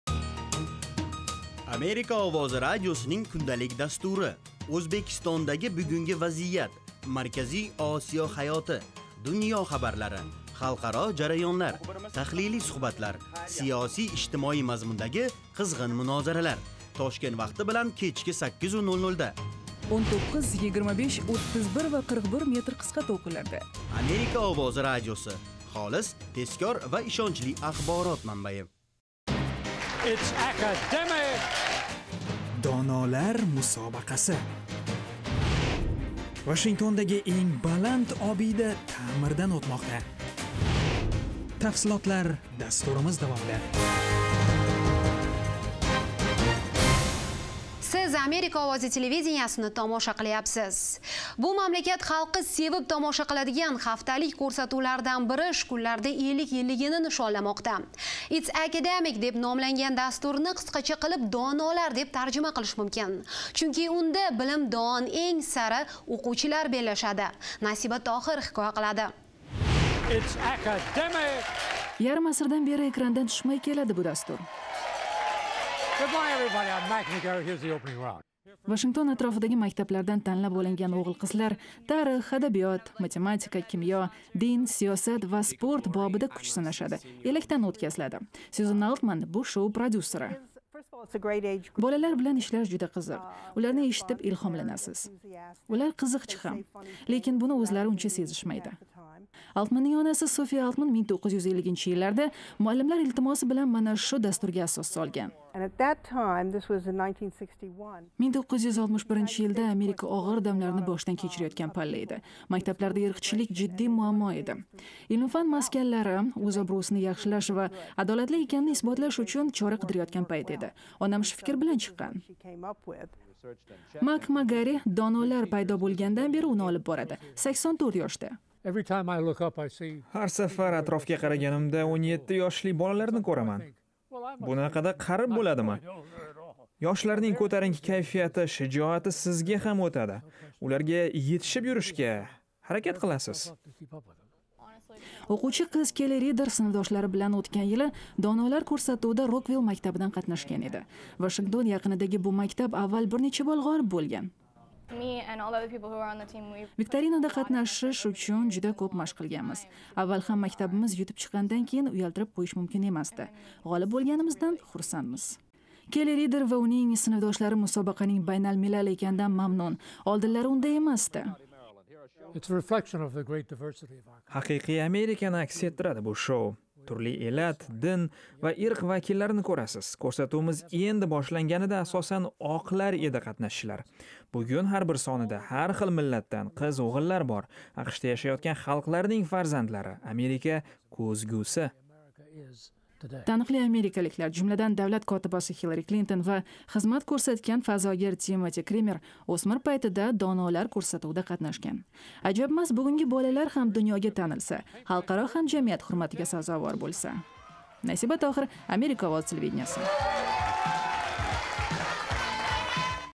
Amerika o'quvchilari bellashadigan dastur/ITS ACADEMIC, 50 YEARS
Amerikaliklar sevib tomosha qiladigan haftalik ko'rsatuvlardan biri shu kunlarda 50 yilligini nishonlamoqda.  "It's Academic" deb nomlangan dasturni qisqacha qilib  "Donolar" deb tarjima qilish mumkin, chunki unda bilimdon, eng sara o'quvchilar bellashadi.